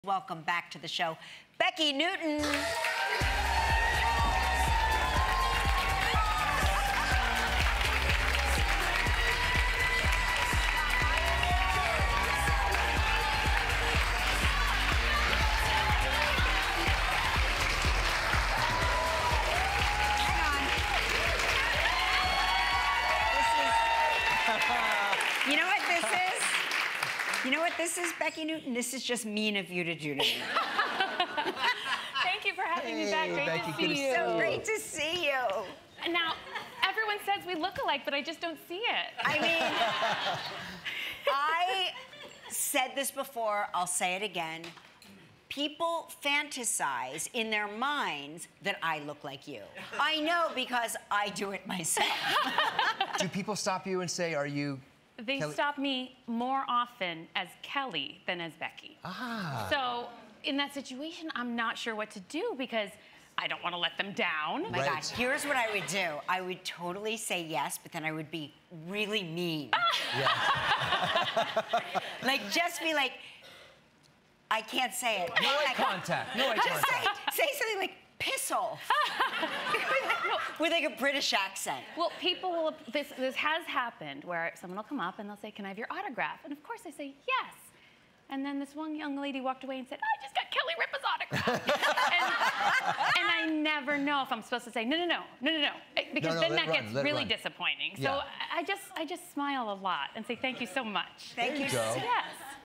Becki Newton and Kelly Are Twinning
Becki Newton dresses like Kelly and talks about getting confused for her all the time.